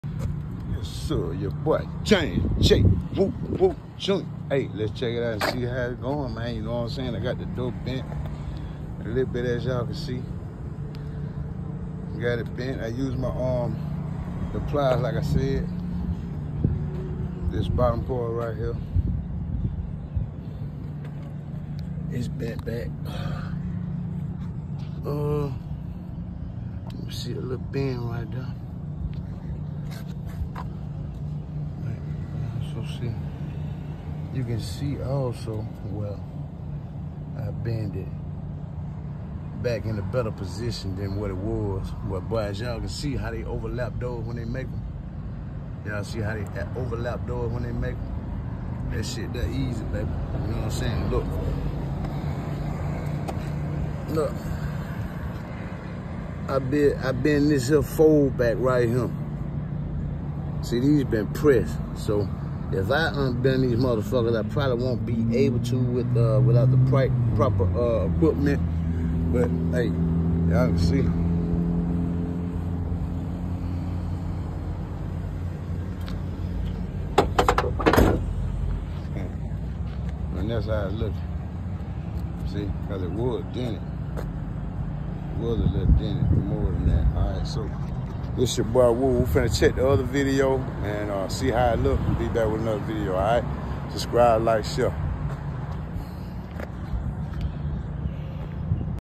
Damage repair after I use some tools to make them and got it looking back normal 😉￼
Damage to my door from a accident that occurred I fixed it using some tools. Got it looking normal. 😉